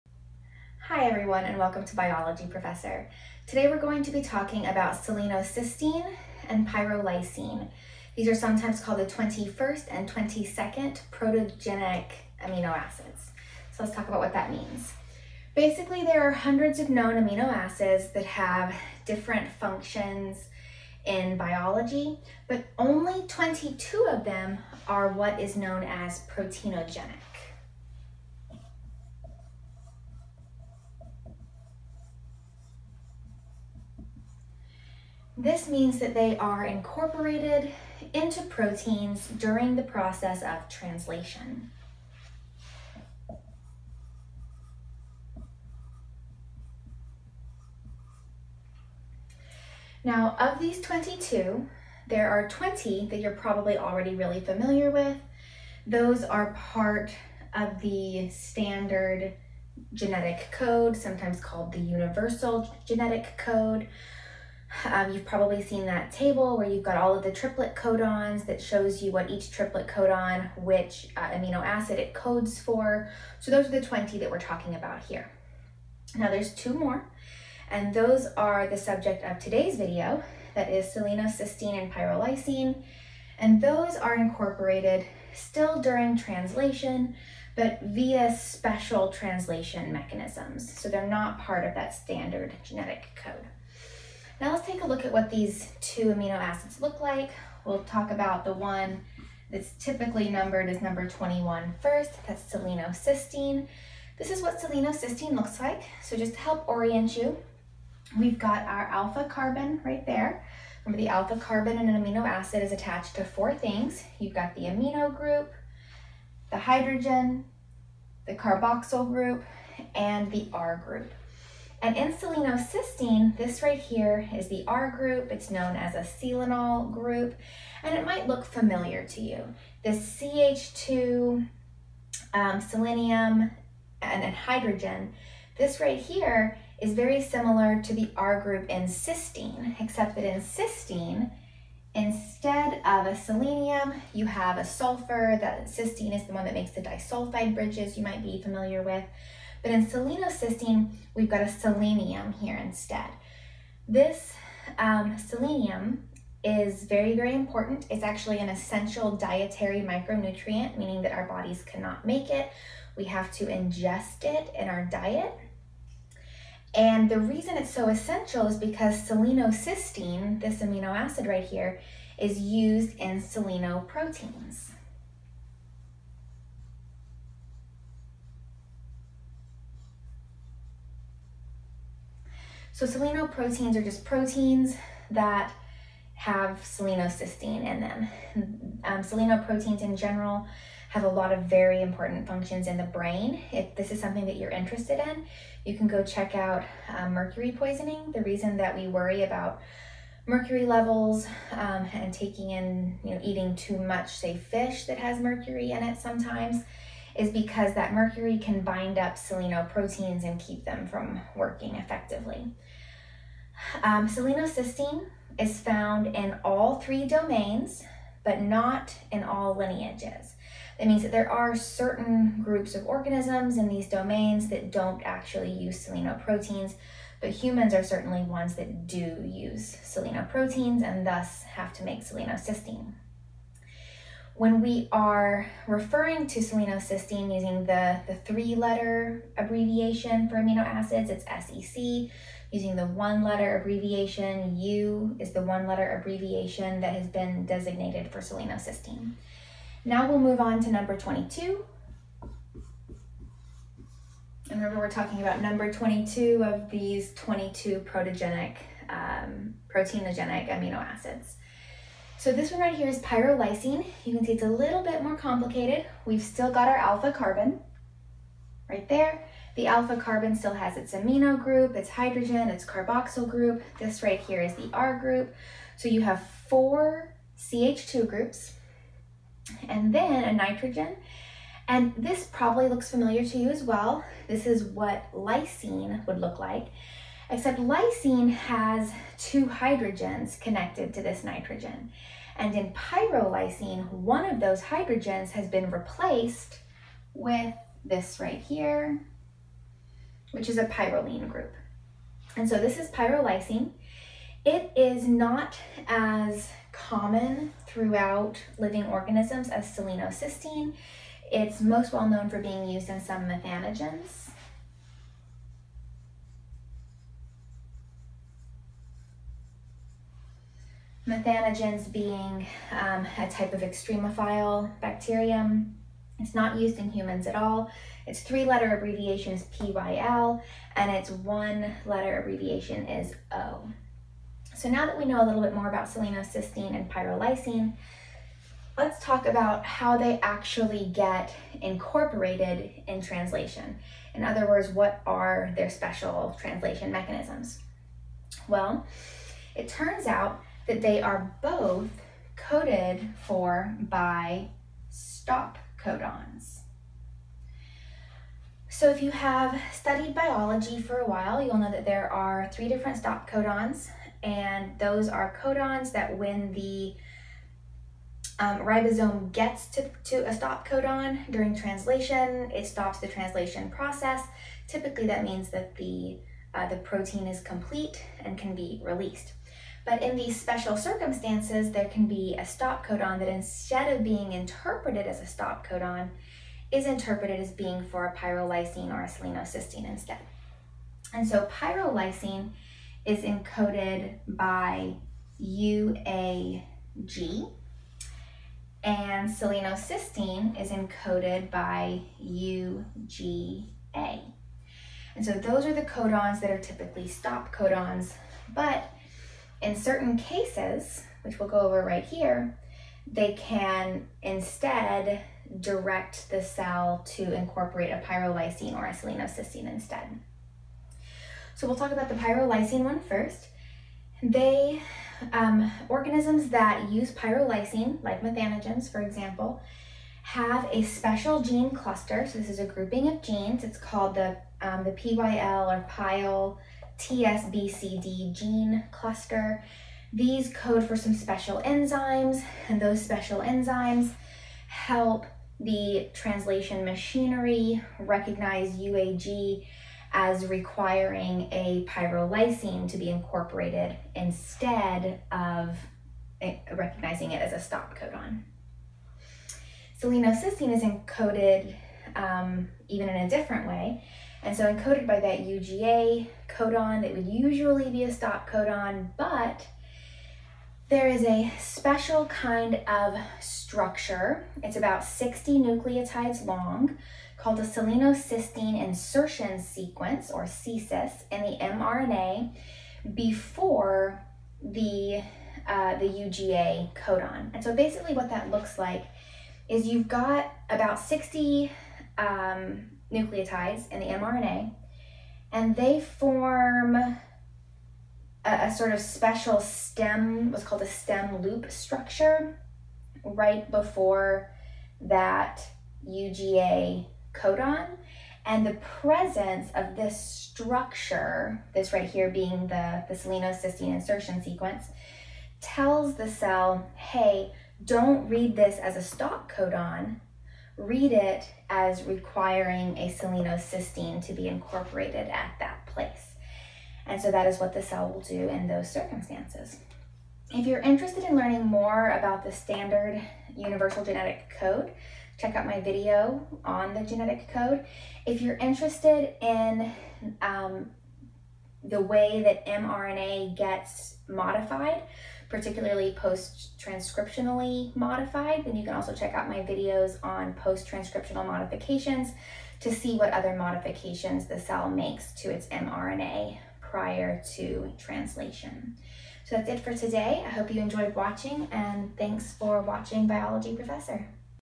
Selenocysteine and Pyrrolysine
In this video, Biology Professor (Twitter: @DrWhitneyHolden) will describe the structures, uses, and insertion mechanisms for the 21st and 22nd proteinogenic amino acids, selenocysteine and pyrrolysine.

Want a PDF of the final white board with all the answers written in?